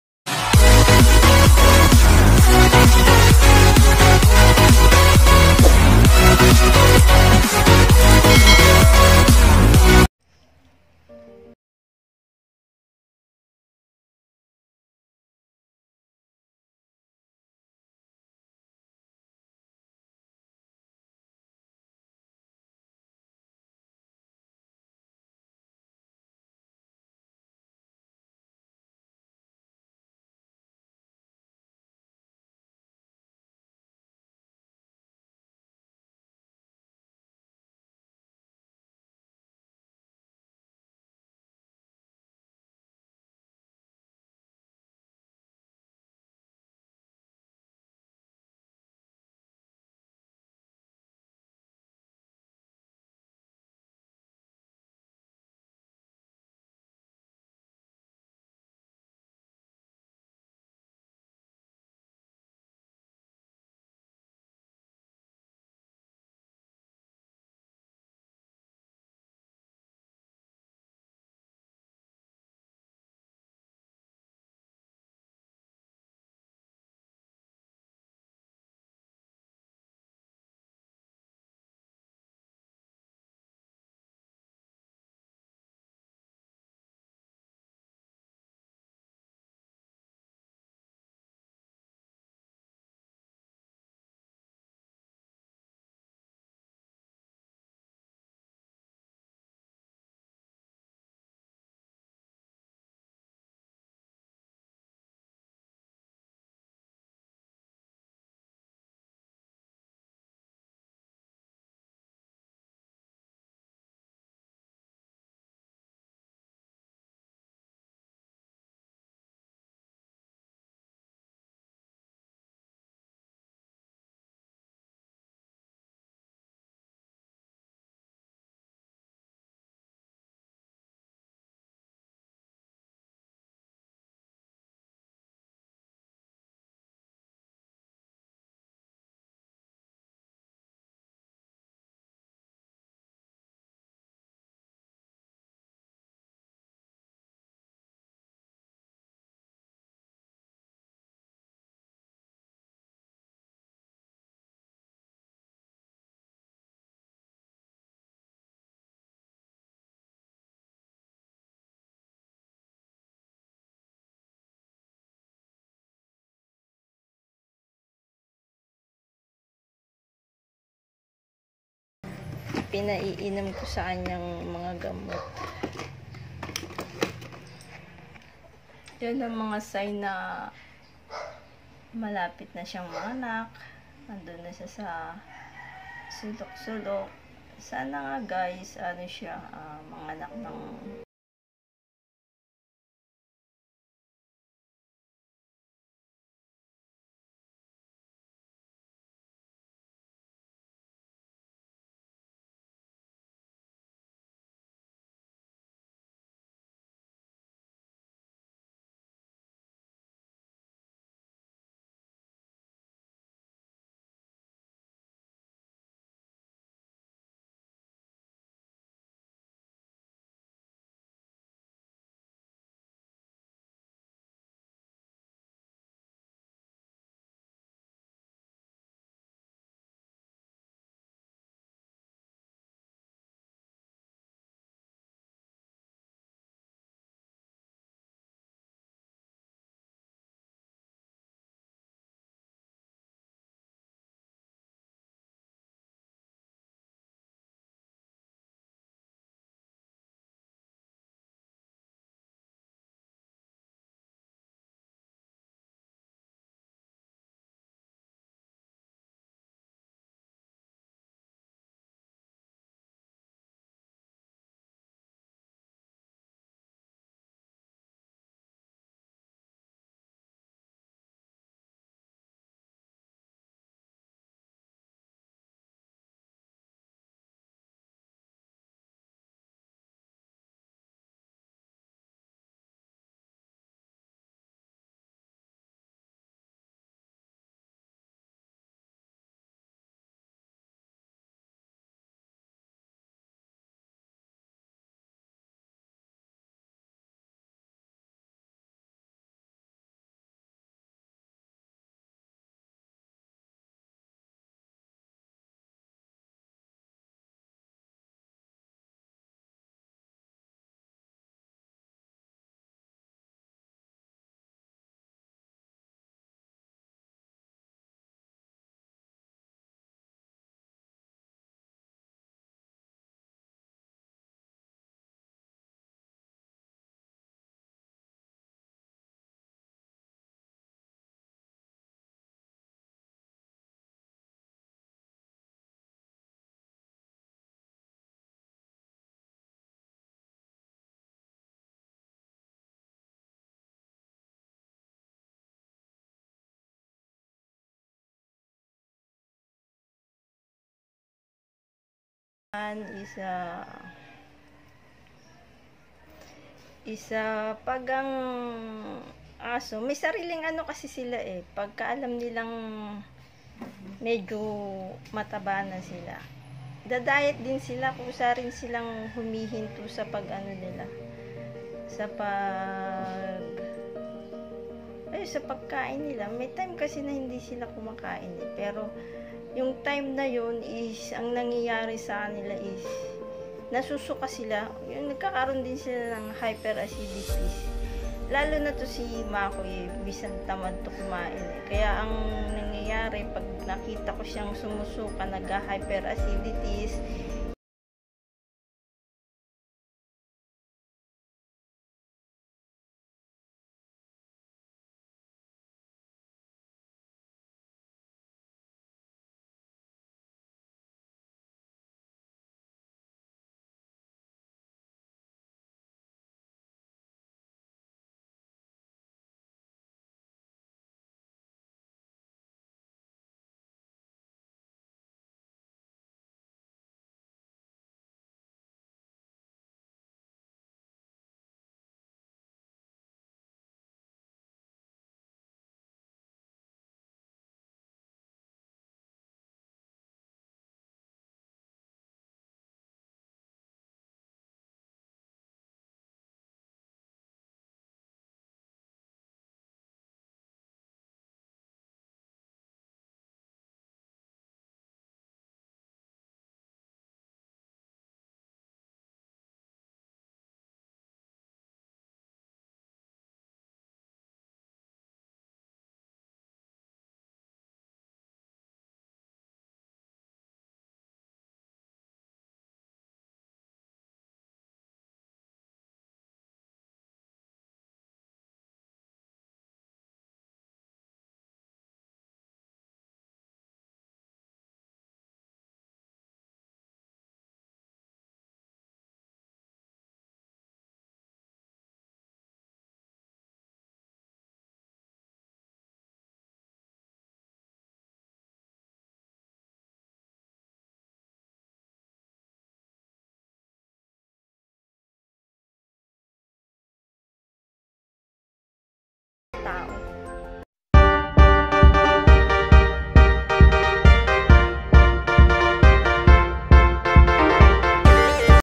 Naka Smile si kitkat (shih tzu) habang waiting manganak.
Malapit na kunting kembot nalang lalabas ba baby ni kitkat,first time mother. shih tzu